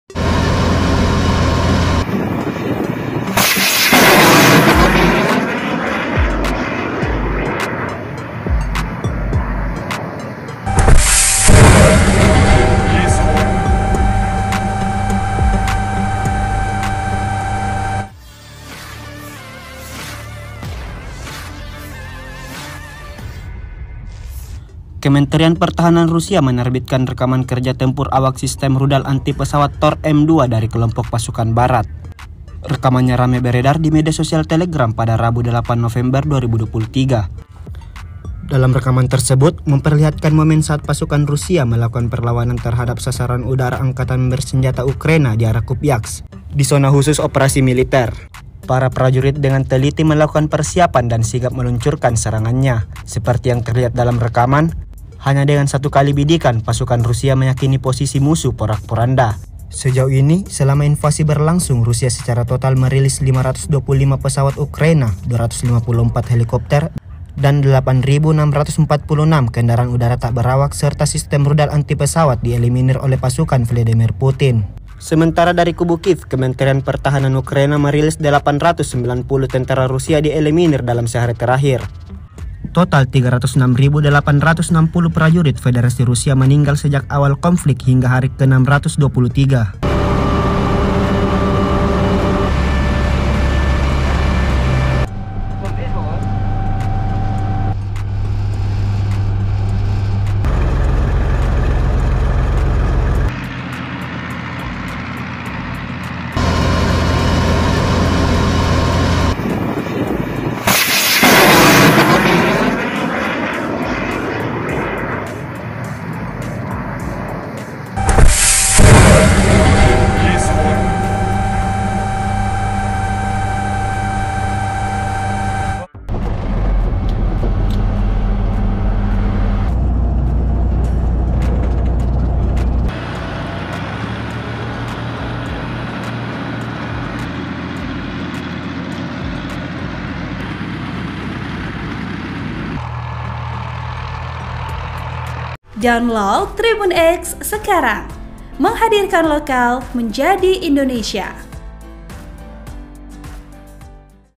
Andalkan Sistem Rudal Anti-Pesawat Tor-M2, Pasukan Rusia Luluh Lantakkan Posisi Ukraina
TRIBUN-TIMUR.COM - Kementerian Pertahanan Rusia menerbitkan rekaman kerja tempur awak sistem rudal anti-pesawat Tor-M2 dari kelompok pasukan Barat.
Dalam rekaman tersebut memperlihatkan momen saat pasukan Rusia melakukan perlawanan terhadap sasaran udara Angkatan Bersenjata Ukraina di arah Kupyansk di zona khusus operasi militer.
Para prajurit dengan teliti melakukan persiapan dan sigap meluncurkan serangannya.
Hanya dengan satu kali bidikan, pasukan Rusia meyakini pososi musuh porak-poranda.
Sejauh ini, selama invasi berlangsung, Rusia secara total merilis 525 pesawat ukraina, 254 helikopter, dan 8.646 kendaraan udara tak berawak serta sistem rudal anti-pesawat dieliminir oleh pasukan Vladimir Putin.

Video Editor: Charly
Naskah: Memet Gunawan
Voive Over: Budi Kurniawan
(Mahasiswa Magang Unismuh)